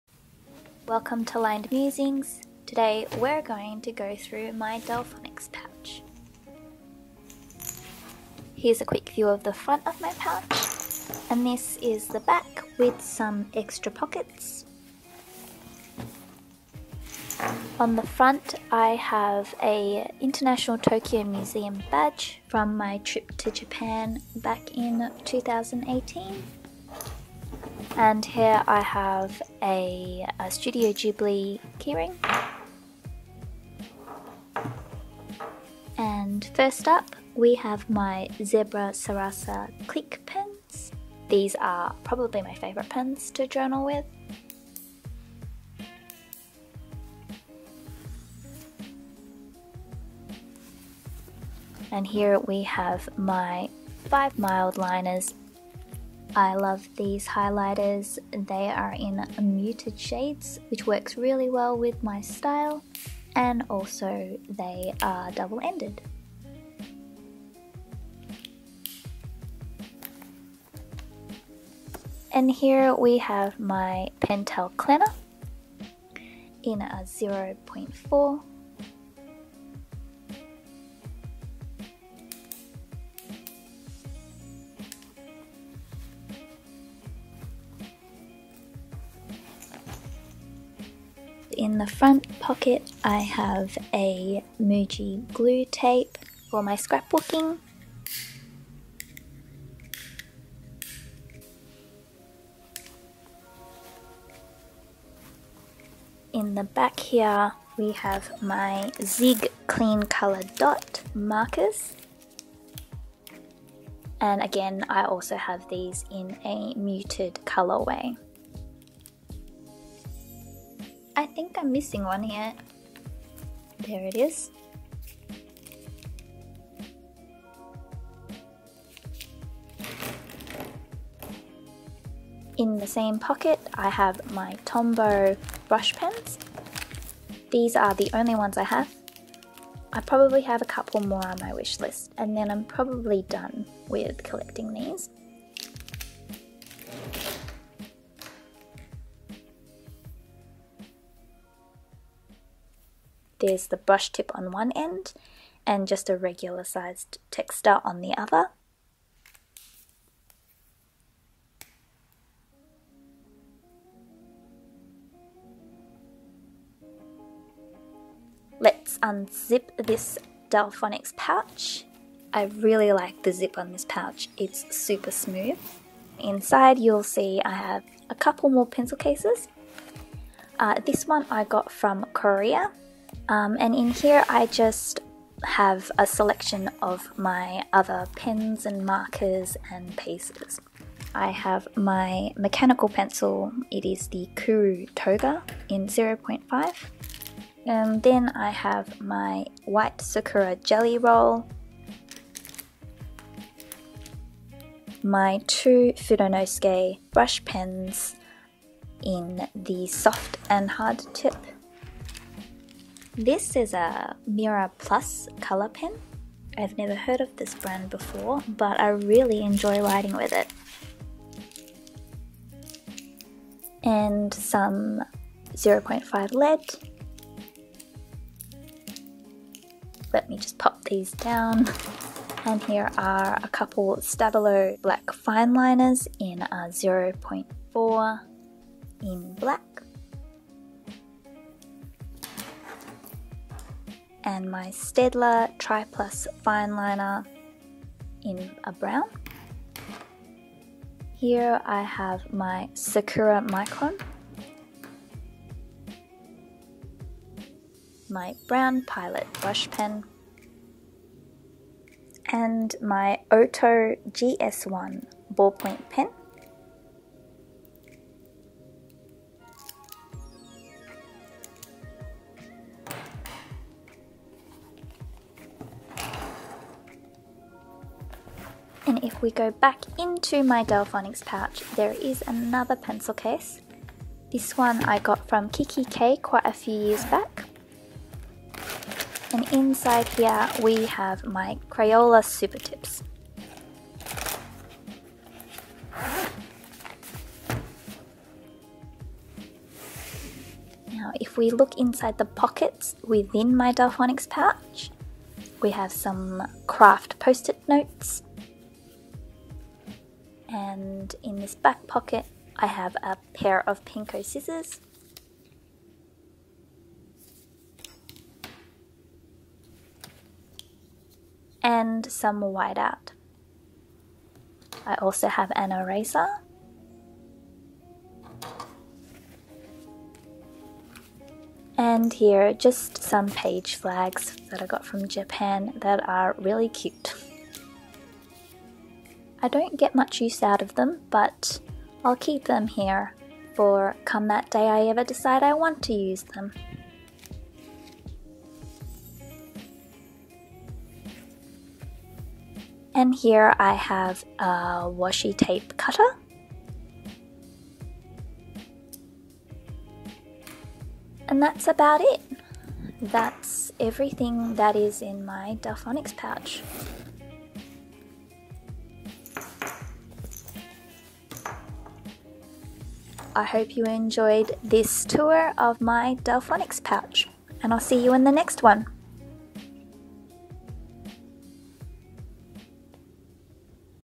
Stationery Pouch Tour - Journalling and Planning Supplies ft. Delfonics Pouch
Here's a little tour of my medium-sized Deflonics pouch. I try to limit my stationery stash to what can fit in this fairly large pencil case, but the stationery world is so vast and tempting - so I can't make any promises. We'll see how I go!

Kuretake ZIG Clean Color Dot Markers in Mild Smoky Set